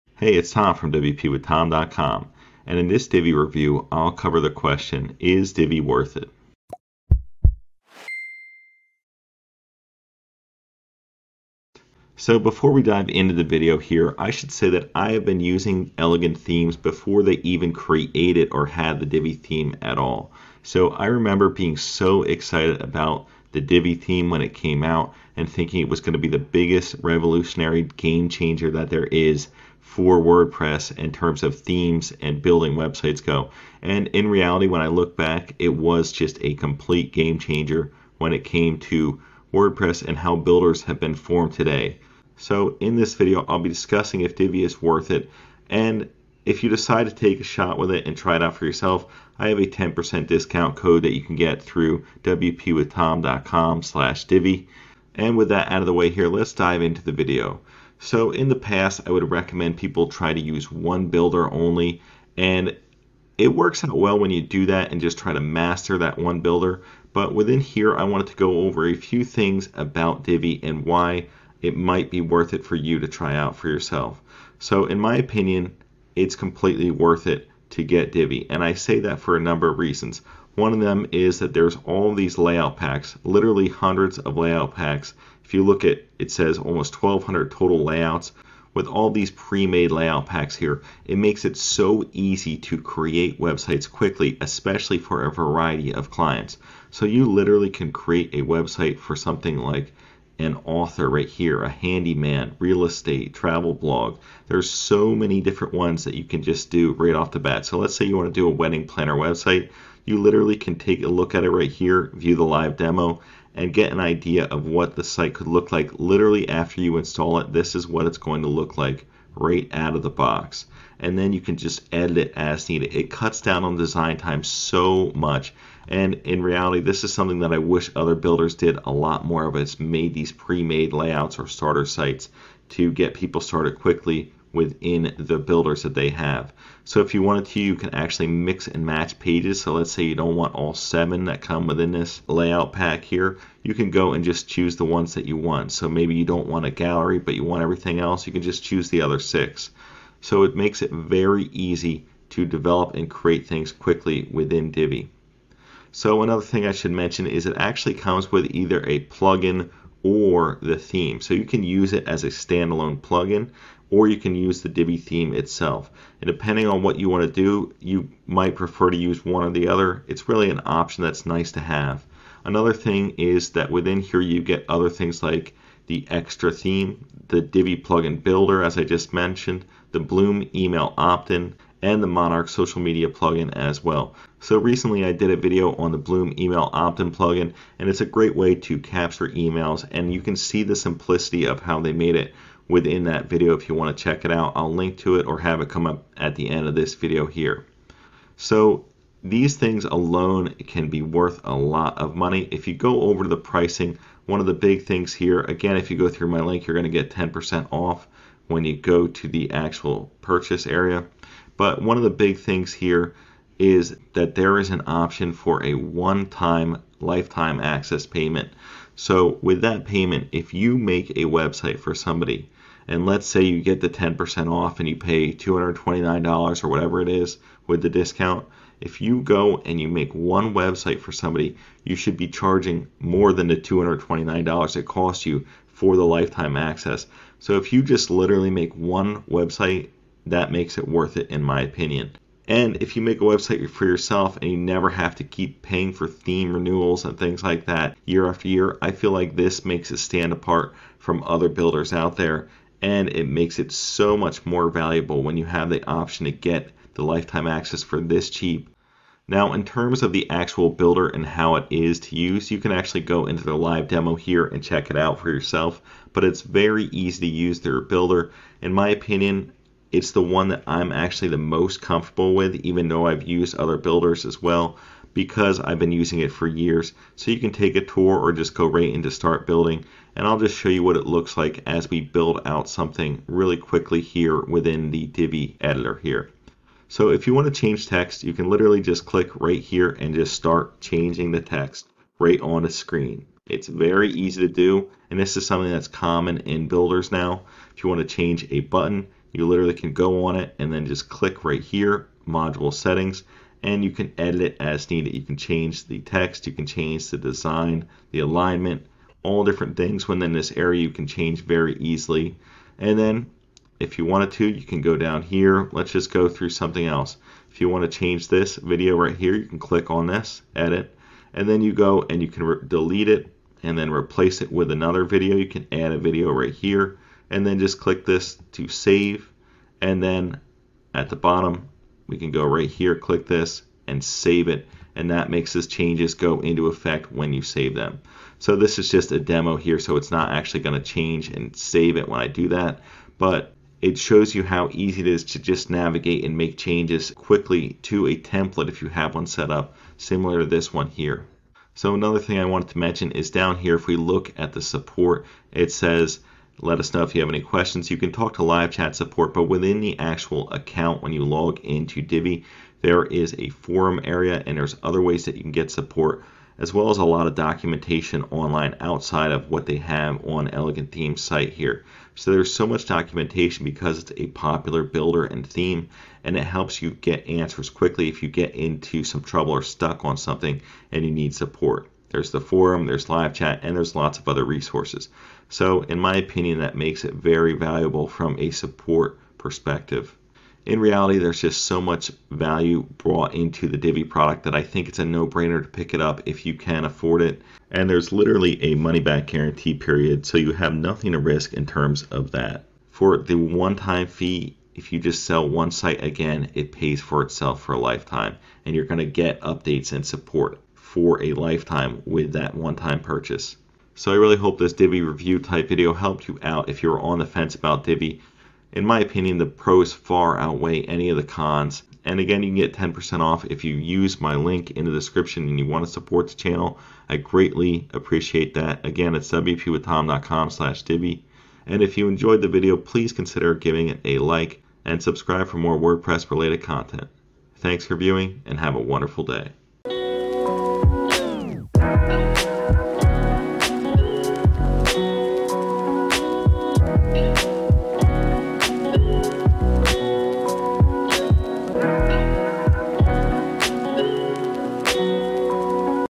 Divi Review : Is Divi Worth it?
Is this Divi review I cover the question is Divi worth it? I've been a member of Elegant Themes for many years, and I've created literally hundreds of websites with Divi. In this review of Divi I explain my thoughts on it, and why I think it's a great option if you can afford it. I hope you enjoy this Divi review video!

Best Hosts:
Hostinger

Other WordPress Builders:
Elementor:

WordPress Theme:
Astra:

SEO:
SEMRush: